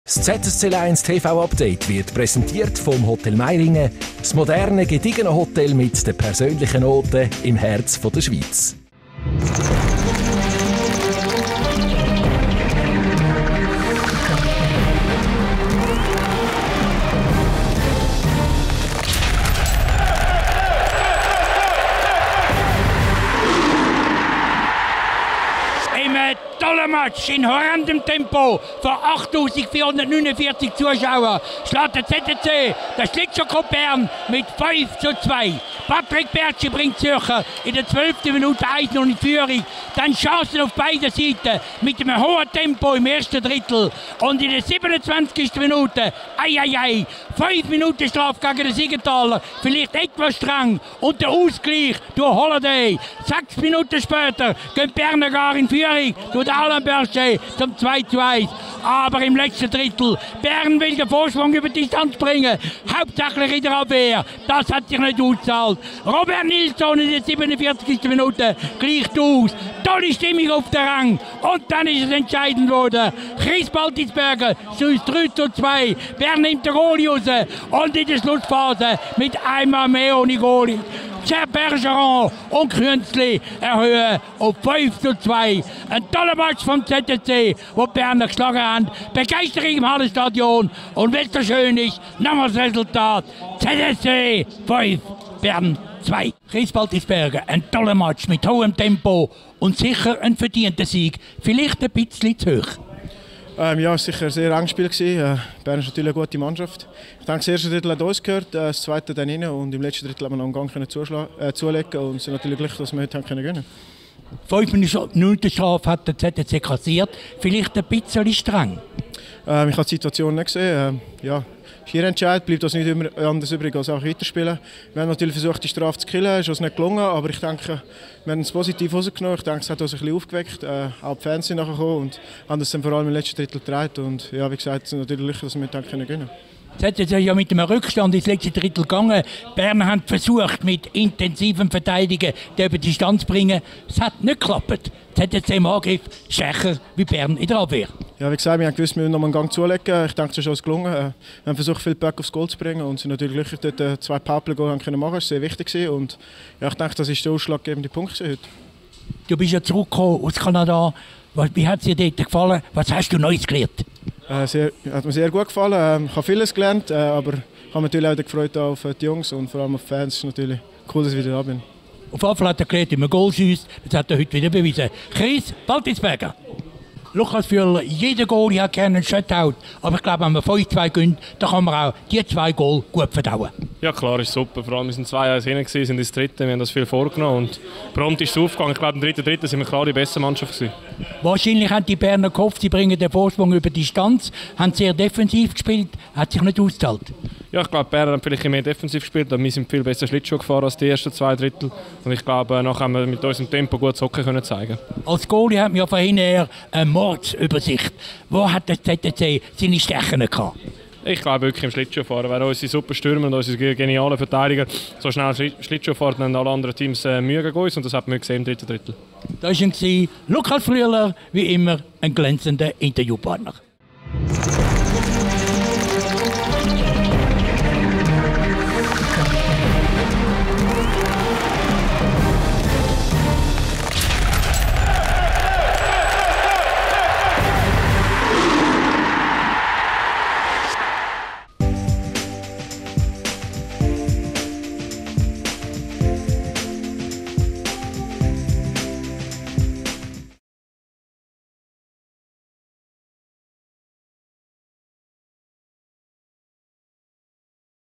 ZSC Lions - SC Bern | Update vom 26.09.2014
Alle Tore und Interviews mit Chris Baltisberger und Lukas Flüeler.